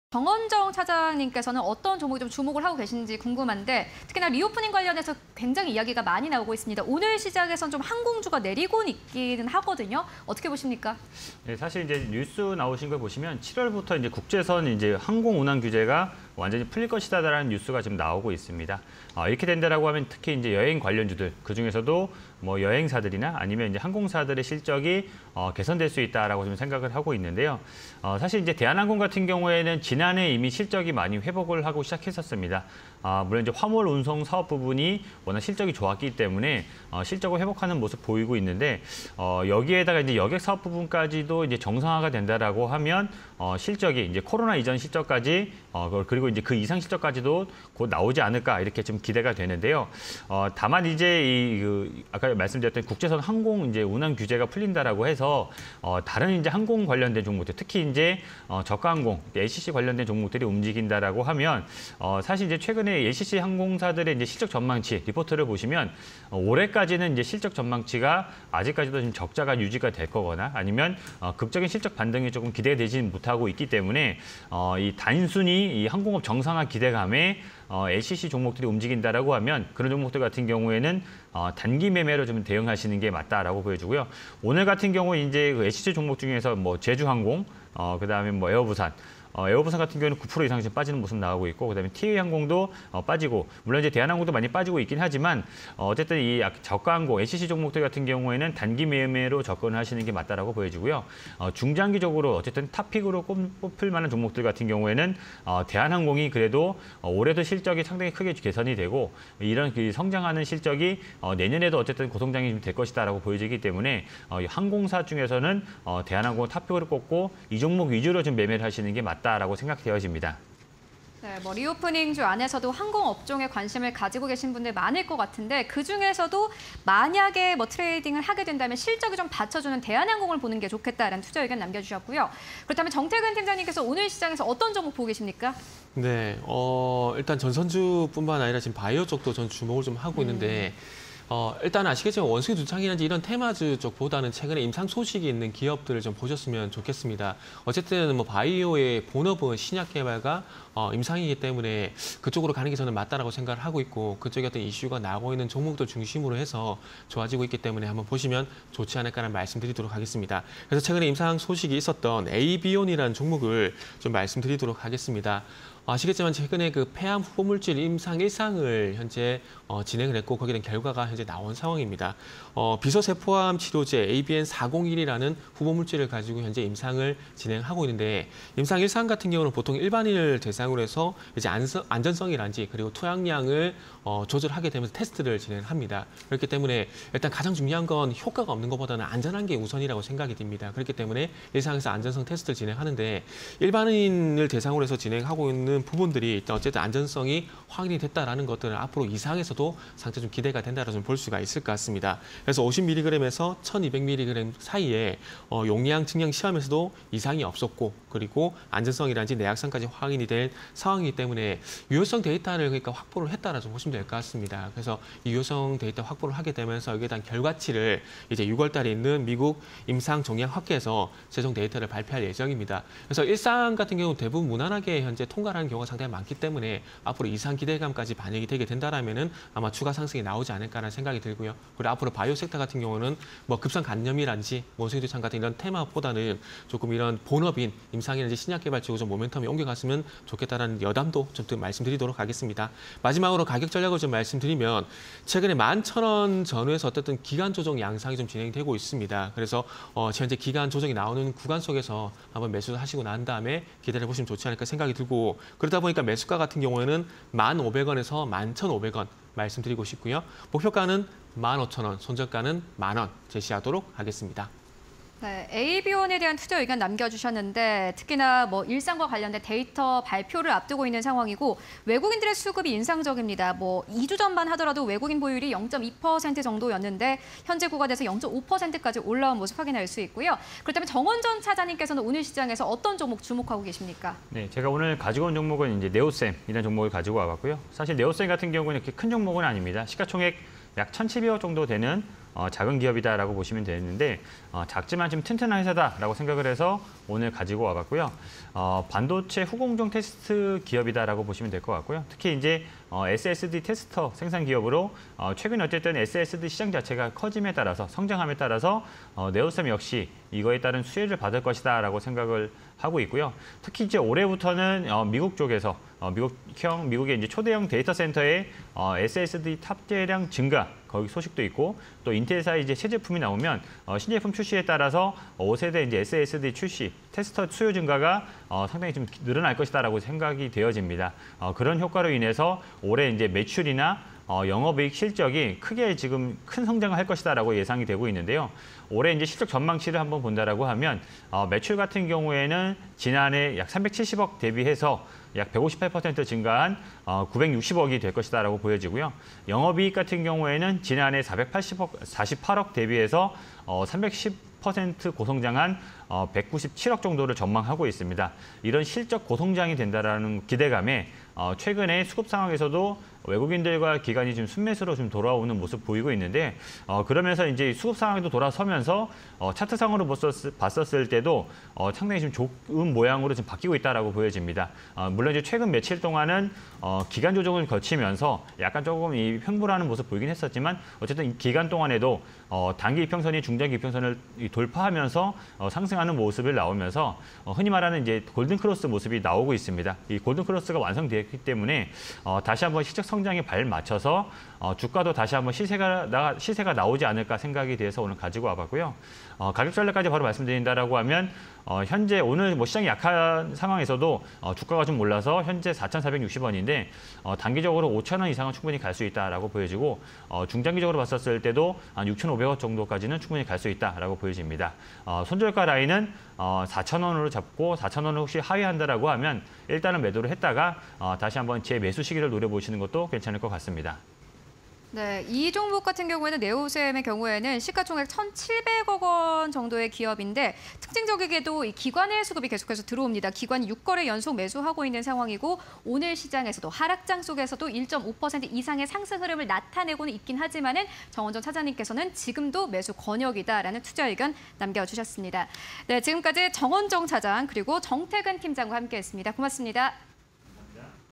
네오셈... 올해 인텔 신제품 출시, SSD 테스터 수요 증가 / 에이비온... 폐암 후보물질 임상 1상, 유효성 데이터 확보_개장 관심주 (20220602)
Q. 항공주, 규제 완화 기대… 주가 날아오를까?
- 7월 항공 운항 규제 완화 기대감
- LCC 실적, 올해까지 부정적 전망
- 항공주 탑픽 '대한항공' 주목

Q. 전선株, 전기차·신재생에너지 확대 수혜?
- 신재생에너지 인프라 + 전기차 확대 수혜
- 안정적 인프라 구축 위한 송배전망 투자 확대
- LS, 대한전선, 일진전기 등 주목

에이비온(203400)
- 폐암 후보물질 임상 1상, 유효성 데이터 확보
- 용량 증량 시험 이상 無… 안정성·내약성 확인
- 6월 美 임상종양학회서 1상 최종 데이터 발표

네오셈(253590)
- 올해 인텔 신제품 출시, SSD 테스터 수요 증가
- 매출 및 영업이익 고성장 예상
- 차트 골든크로스 발생… 추가 상승 전망